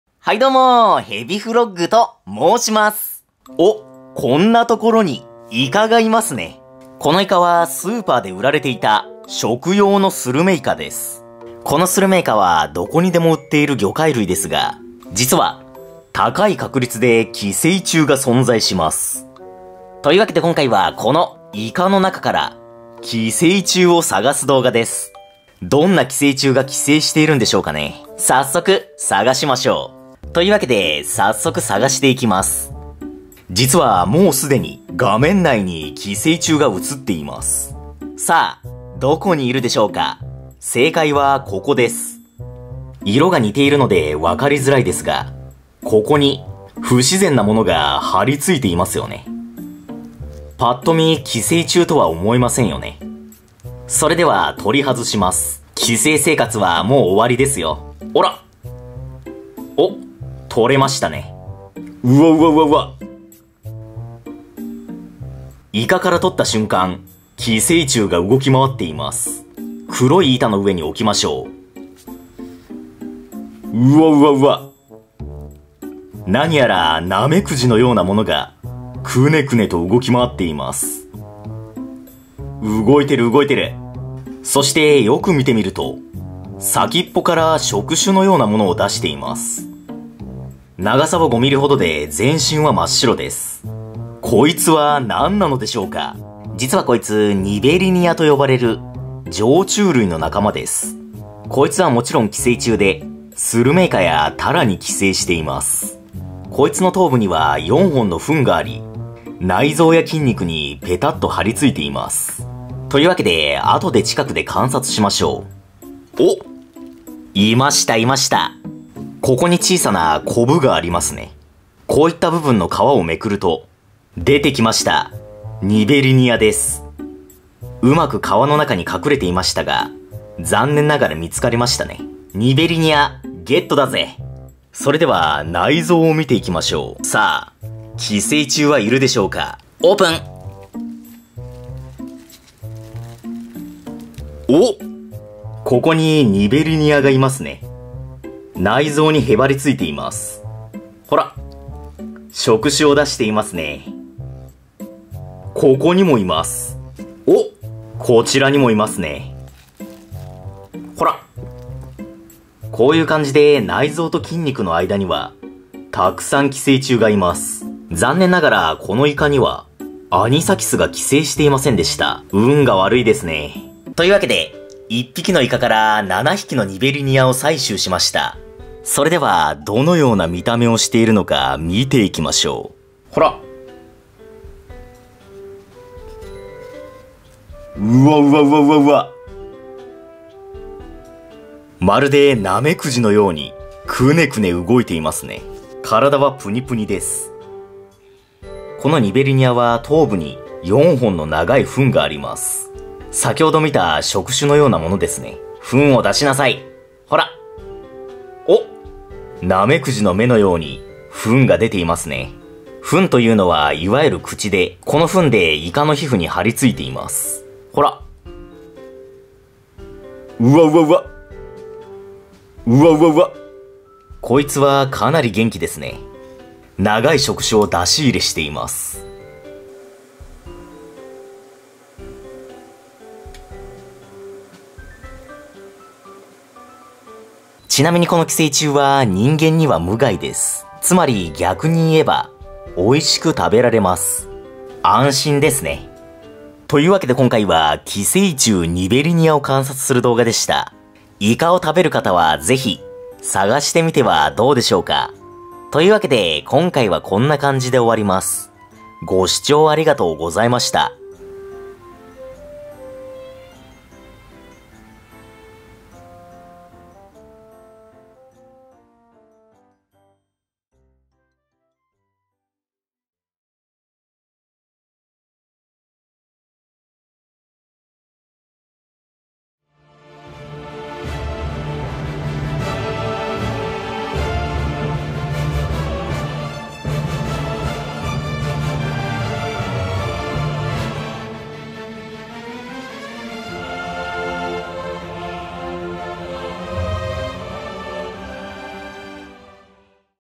スーパーで売られていたイカから大量の寄生虫が…
イカリングにしました